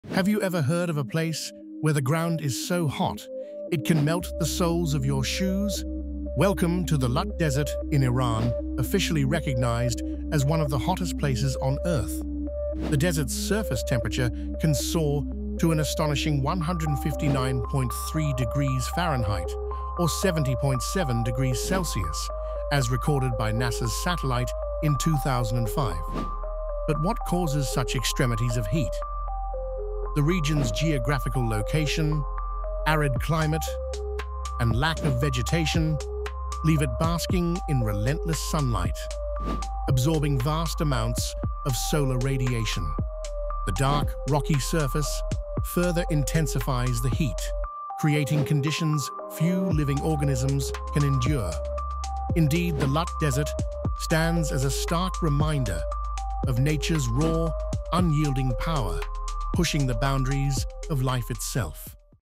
🔥 The Hottest Place on Earth – Lut Desert’s Extreme Heat! 🌡️
Have you ever heard of a place where the ground is so hot it can melt the soles of your shoes? 🌍🔥 Welcome to the Lut Desert in Iran, officially recognized as one of the hottest places on Earth. With surface temperatures soaring to 159.3°F (70.7°C), this barren landscape is a true test of nature’s extremes. But what makes it so incredibly hot? ☀️

🏜️ The geographical location, arid climate, and dark rocky terrain trap immense heat, making survival nearly impossible for most living organisms. Join us as we explore NASA’s discoveries and the science behind this blazing inferno on Earth!